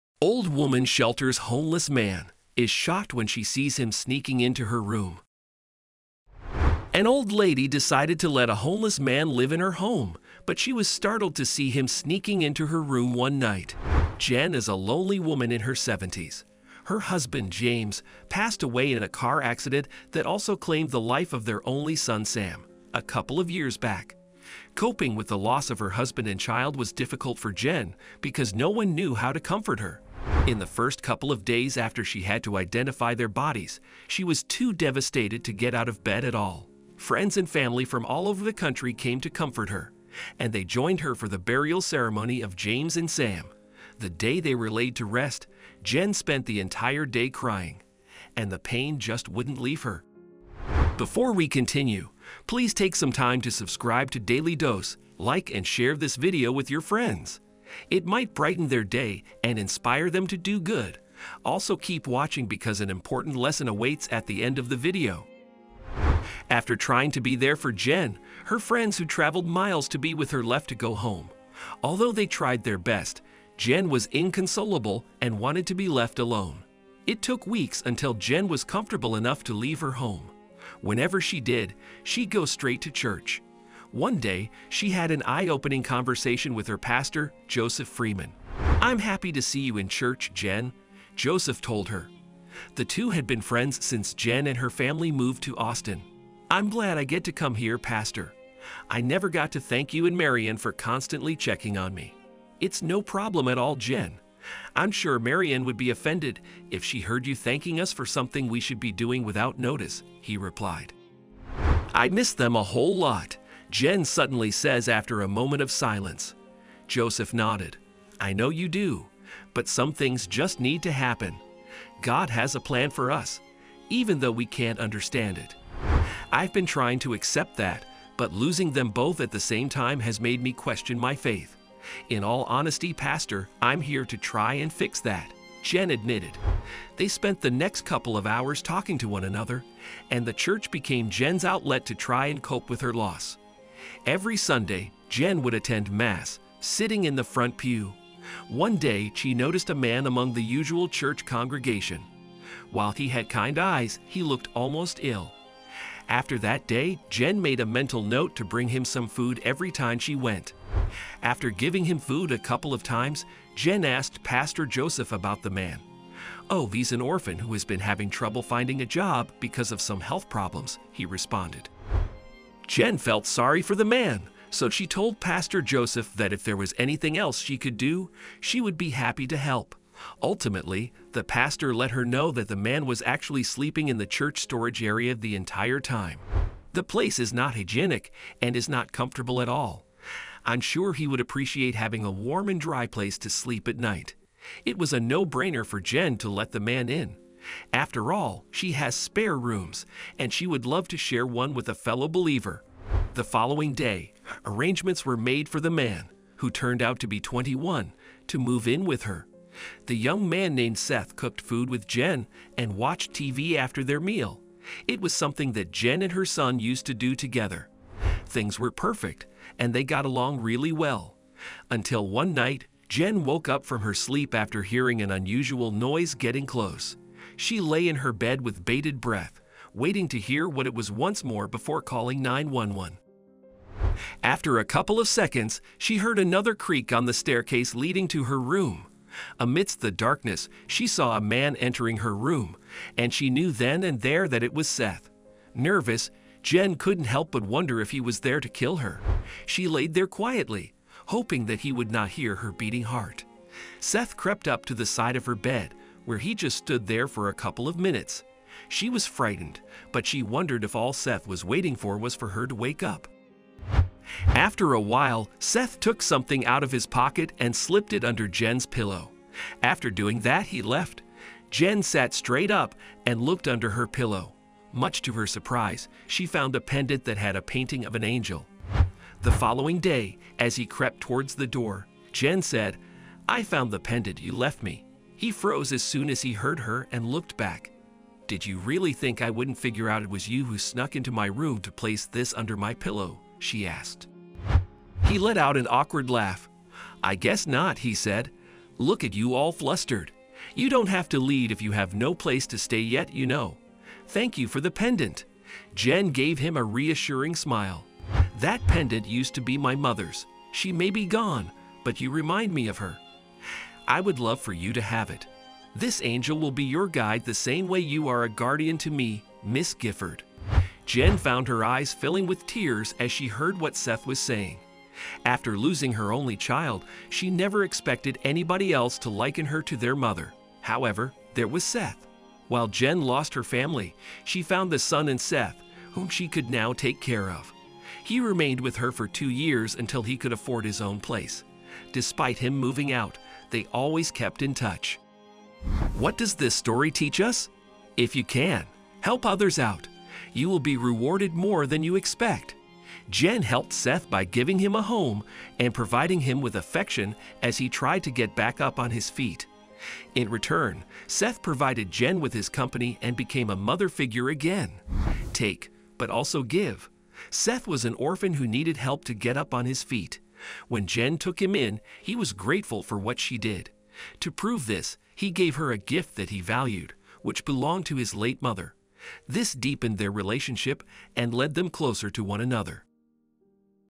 Old Woman Shelters Homeless Man, Is Shocked When She Sees Him Sneaking Into Her Room
An old lady decided to let a homeless man live in her home, but she was startled to see him sneaking into her room one night.